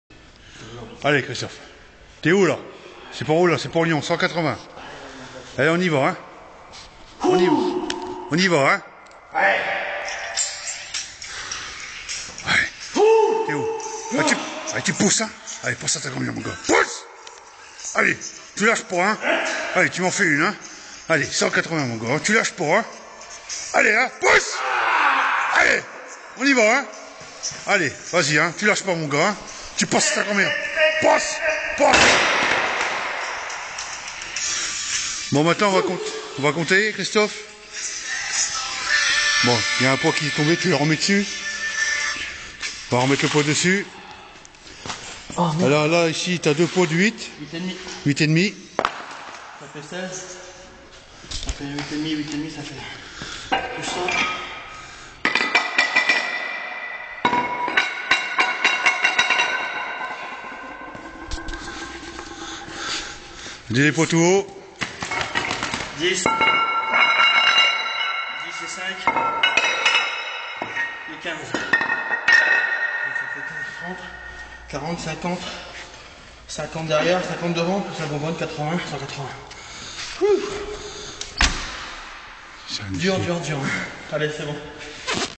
Farmer walk 180kg (marche du fermier 180kg) Christophe
quelques metres avec la marche du fermier a 180kg dans chaque main